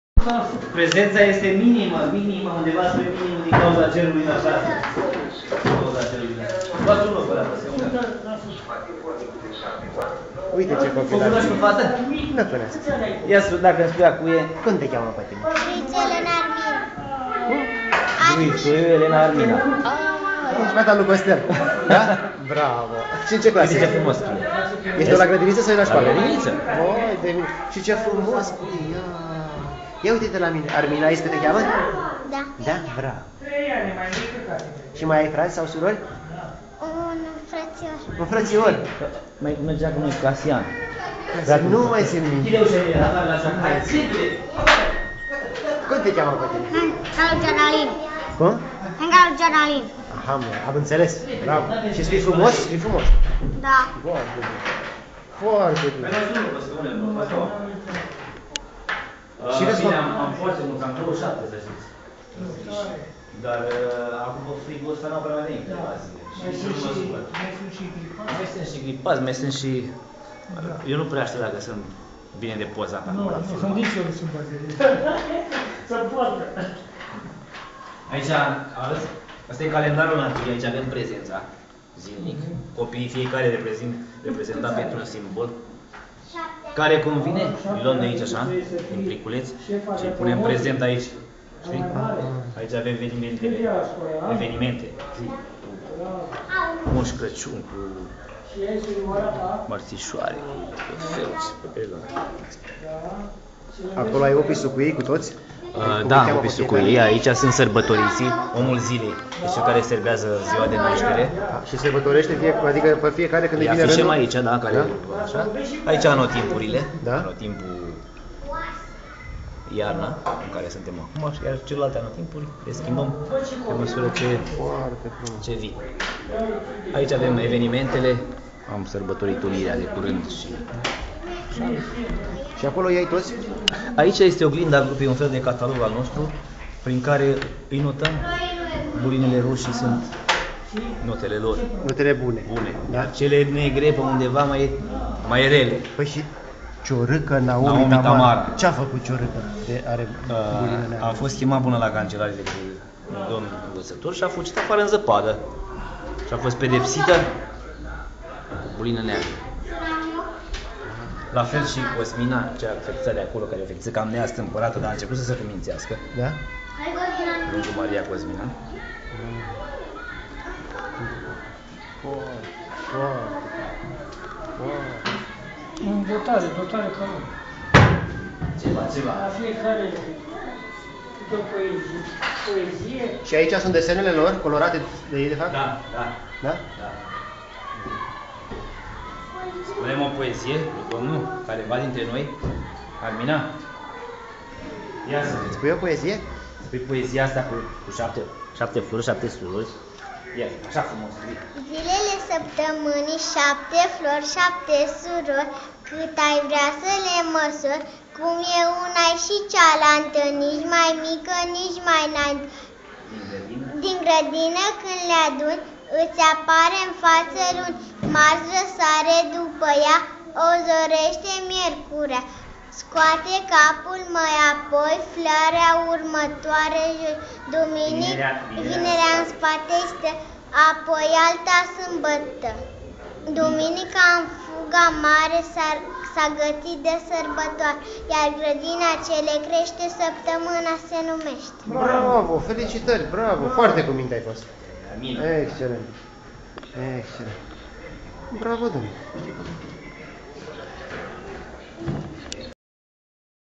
Gradinita Madulari-Cernisoara-Valcea
Gradinita Madulari-Cernisoara-Valcea, ianuarie 2010. Vorbeste  Daniel Smedescu, educator.